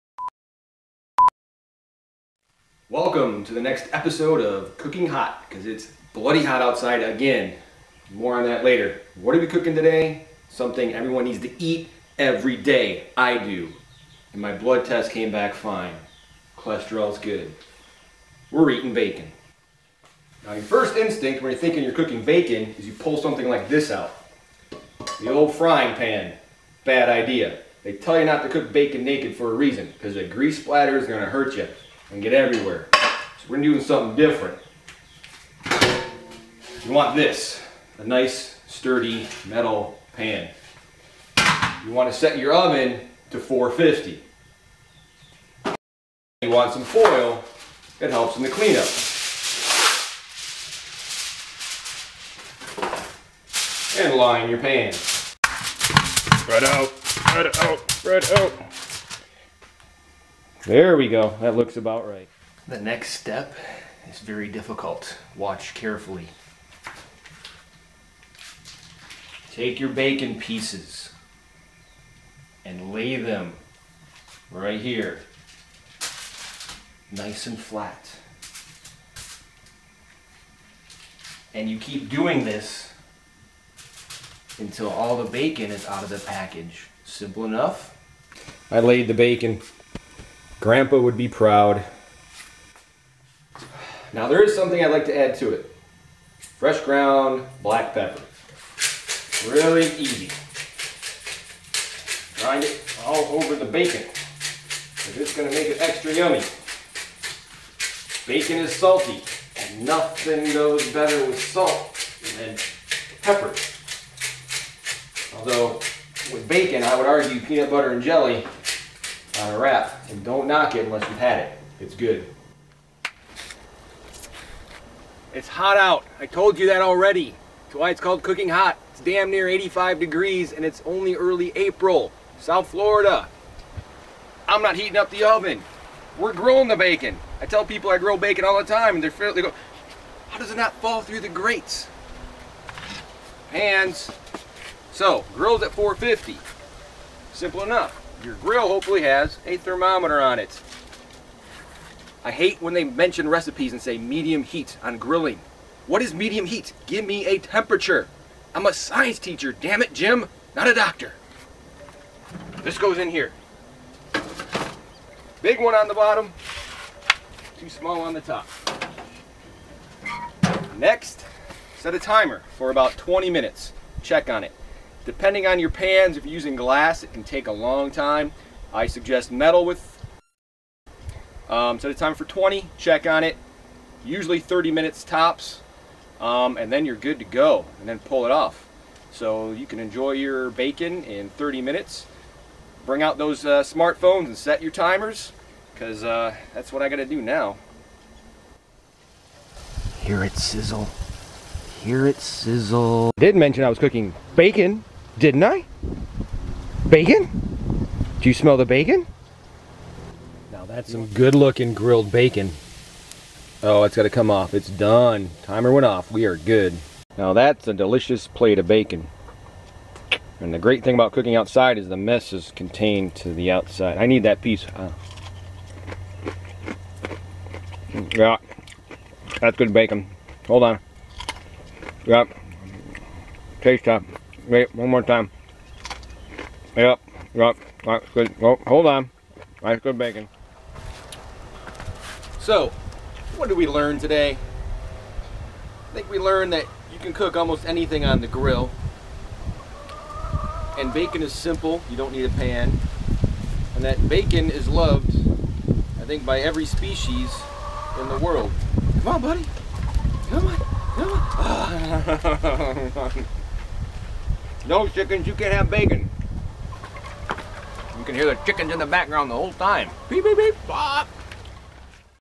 Cooking HOT...The BACON episode!
This is more of my new show, Cooking HOT! This time, it's bacon, the easy way...on the grill, duh!